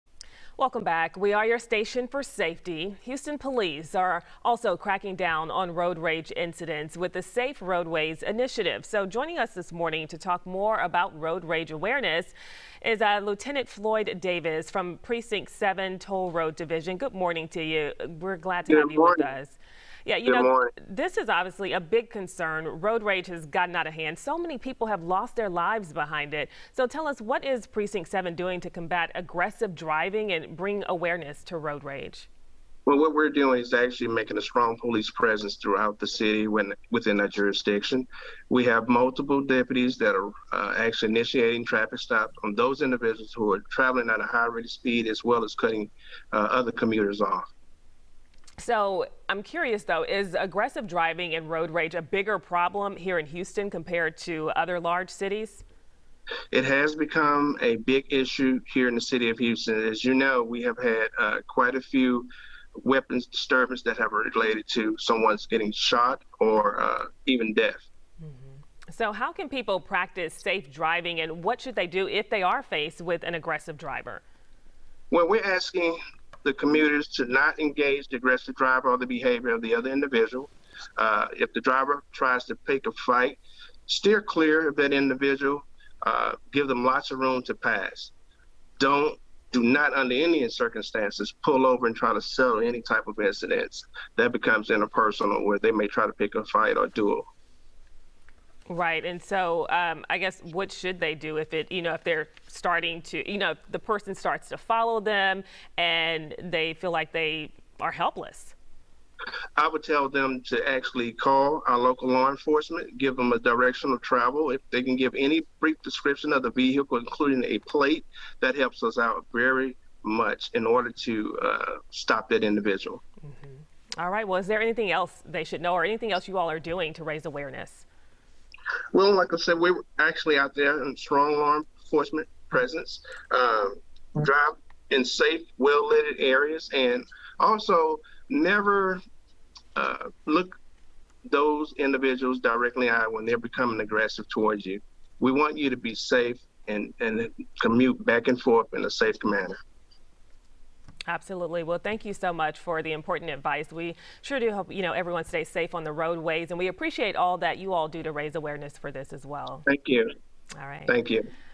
Avoiding road rage incidents
Authorities in the Houston area are cracking down on road rage with the Safe Roadways Initiative. Lt. Floyd Davis with the Precinct 7 Toll Road Division talks about steps being taken to address road rage incidents.